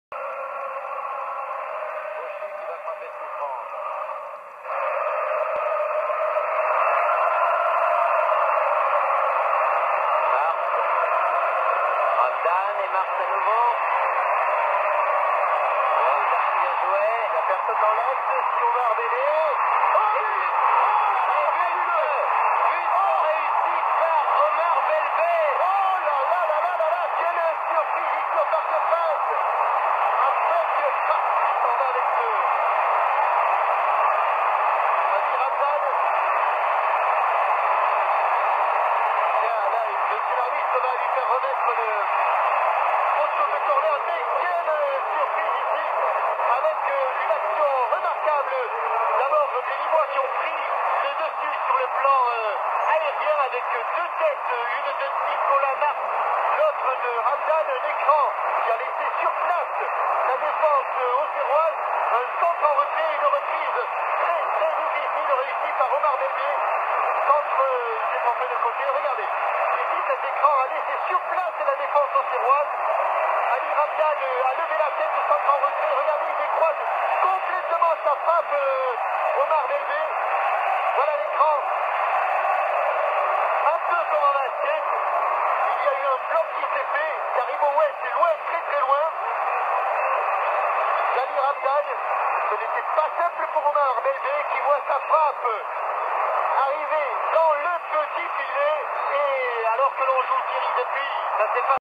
But Belbey
But d'Omar Belbel en finale de la coupe de France 1996, NO vs AJA